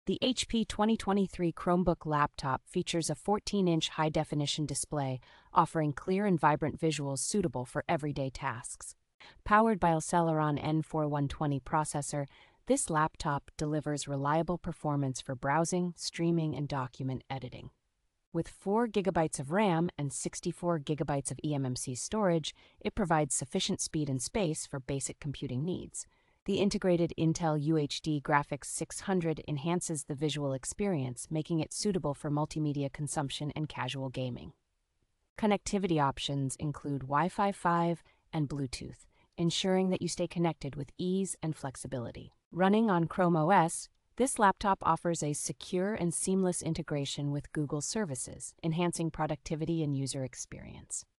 REVIEW (2024): HP 2023 Chromebook Laptop. ESSENTIAL details.
The HP 2023 Chromebook Laptop features a fourteen inch high definition display, offering clear and vibrant visuals suitable for everyday tasks.

Powered by the Intel Celeron N4120 processor, this laptop delivers reliable performance for browsing, streaming, and document editing.

With four gigabytes of RAM and sixty-four gigabytes of eMMC storage, it provides sufficient speed and space for basic computing needs.

The integrated Intel UHD Graphics 600 enhances the visual experience, making it suitable for multimedia consumption and casual gaming.

Running on Chrome OS, this laptop offers a secure and seamless integration with Google services, enhancing productivity and user experience.